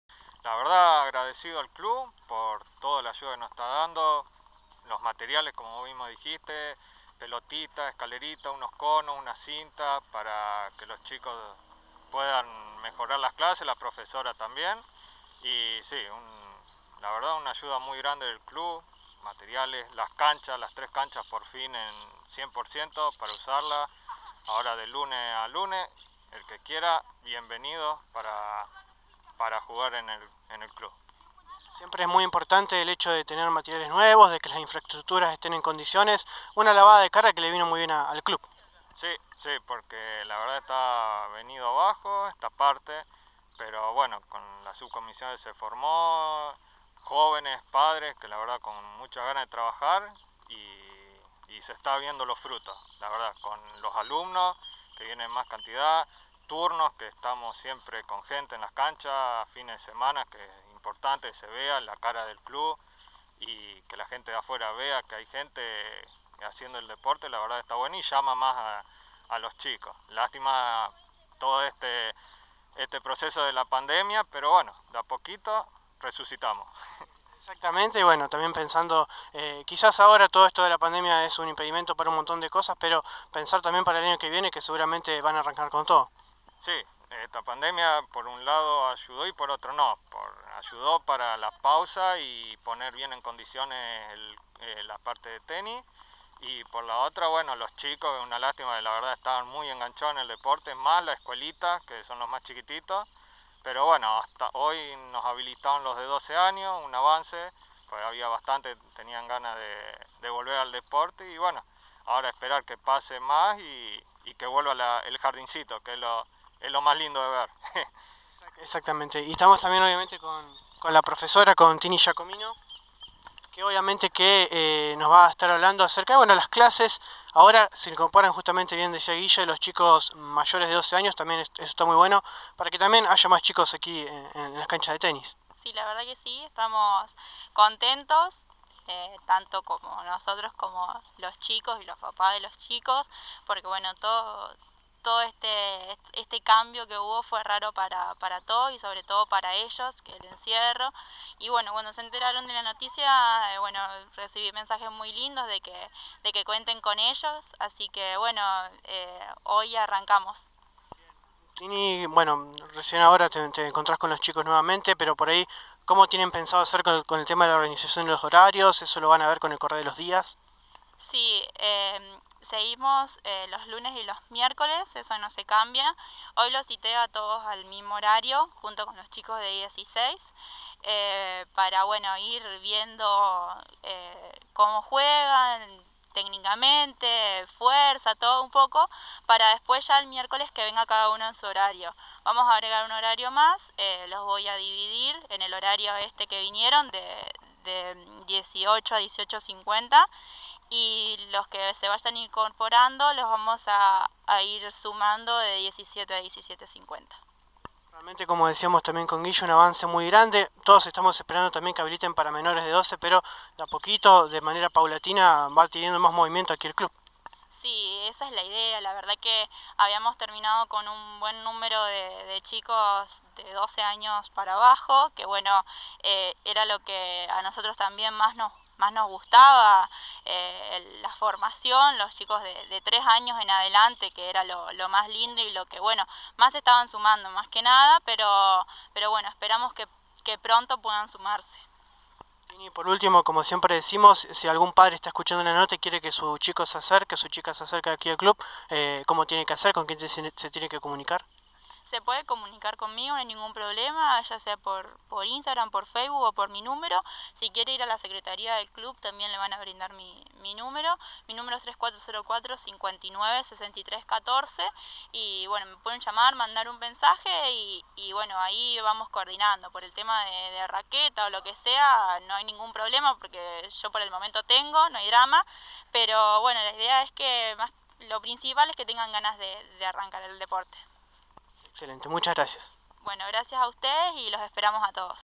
Guillermo Tourn - Tini Giacomino - Tenis C.A.A.
Recibieron nuevos materiales para las prácticas - Regreso de los mayores de 12 años a las prácticas